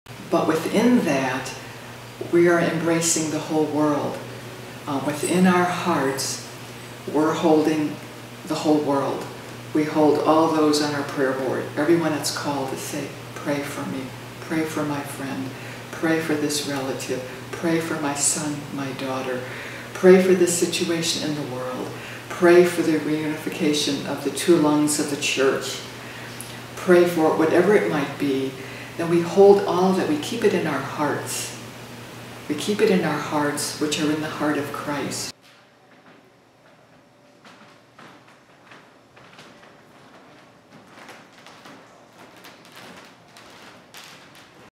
'Pray To Love' Cloistered Living Episode 2
While the world is becoming quarantined, I would like to share a viewpoint of being, for over 400 years, 'happily quarantined'. These women are the cloistered, the contemplatives, the seekers. We can all learn from the Visitation Nuns to help us during these times. Part 2.